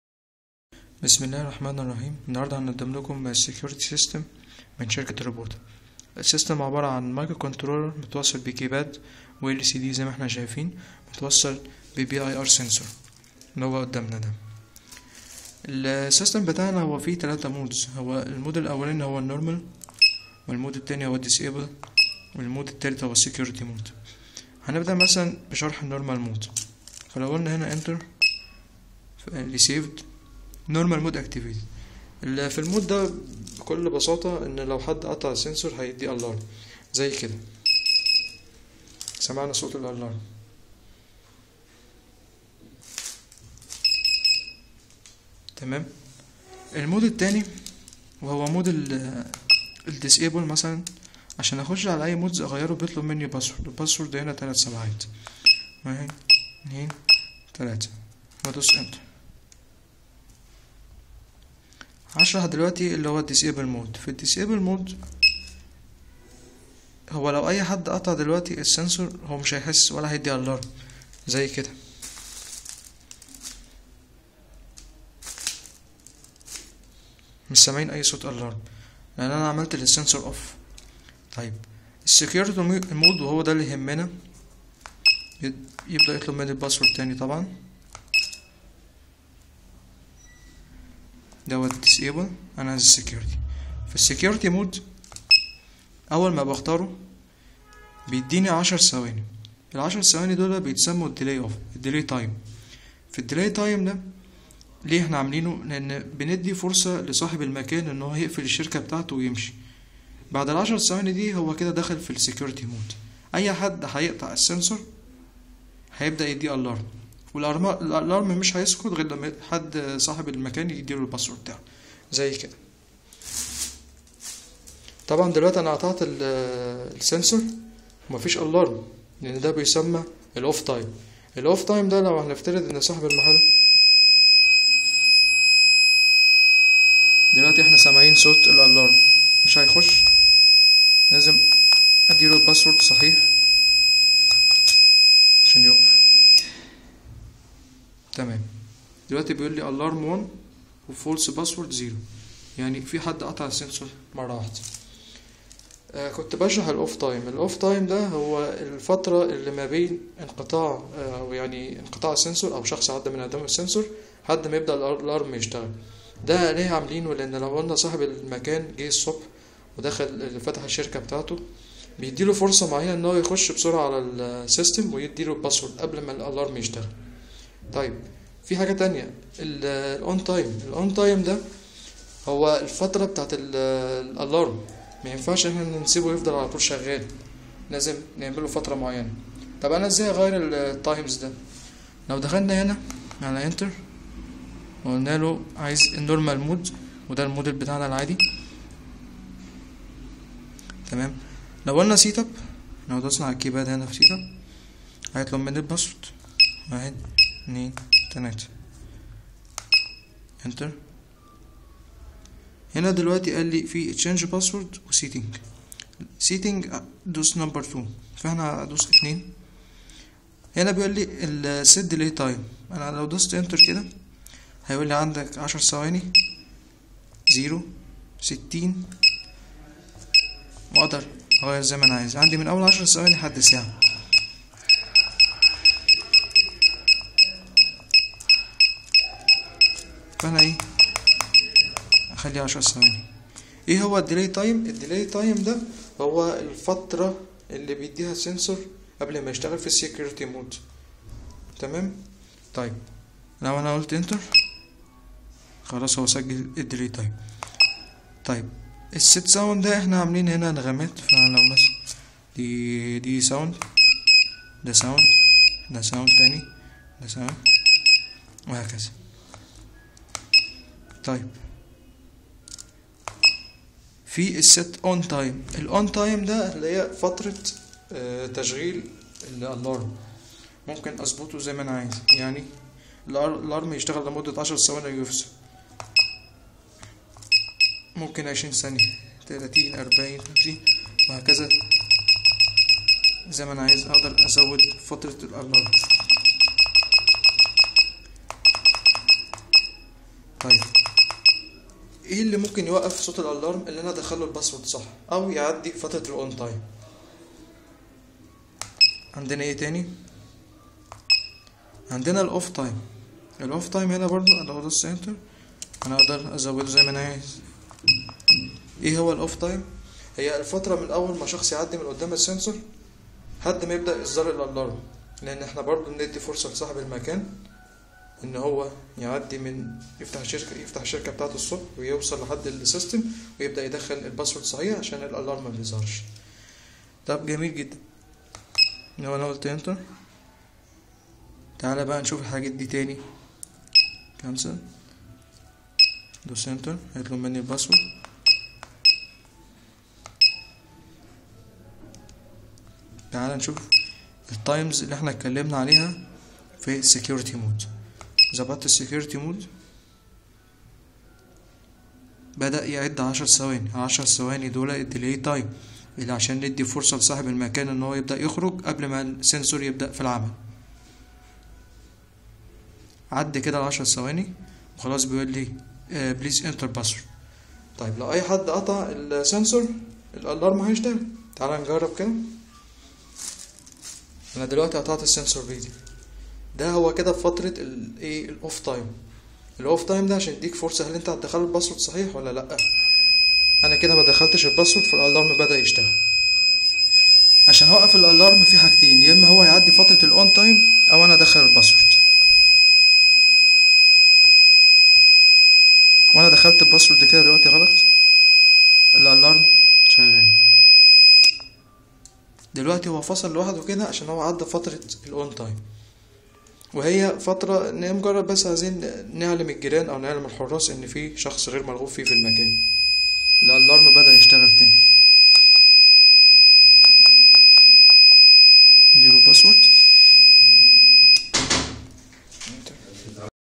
Robota Security System.mp4
Robota Security System , for small companies & offices, the alarm is fired ON if any strange one enter the office, this alarm will be turned OFF after a certain adjustable time or after entering the correct password ,,,,

see the video & have fun :)